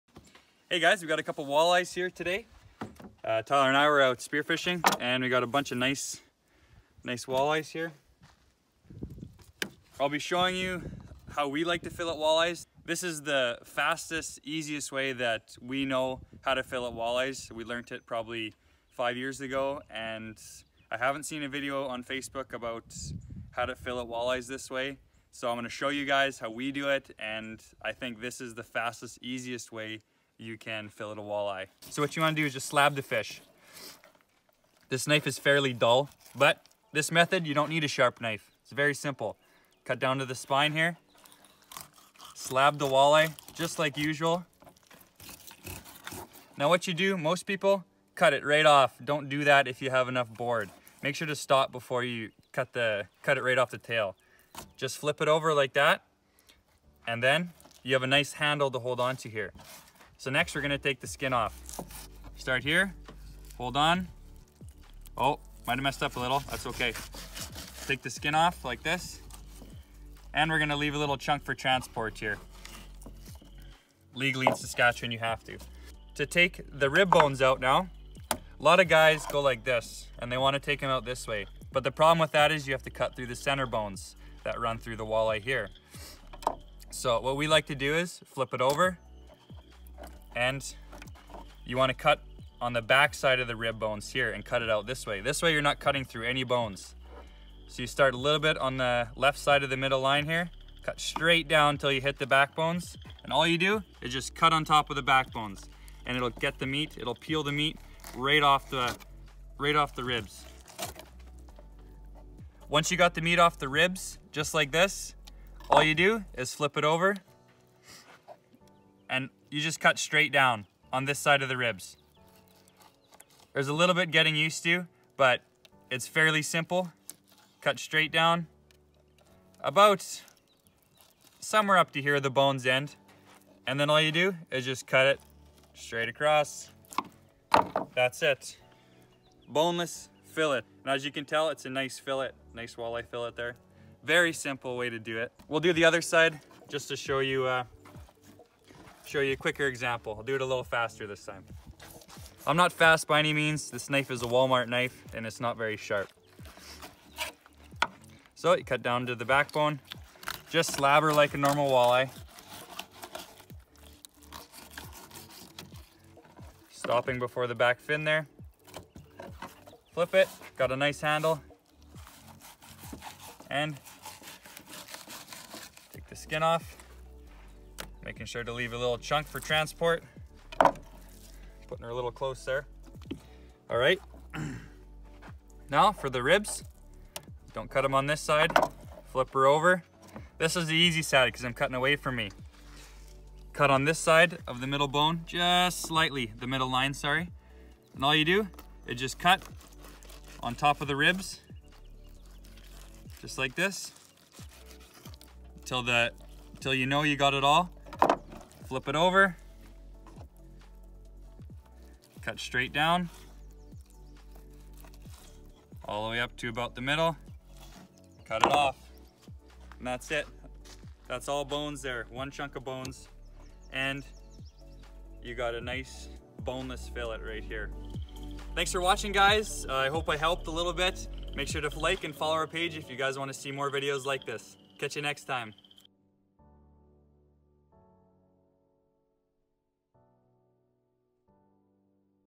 How to FILLET a WALLEYE - NEW METHOD!
How to fillet a walleye and a new method different from any other video out there!

Cleaning a walleye does not have to be a tricky thing to do. We will be showing you how to fillet a walleye the easiest and fastest way you can!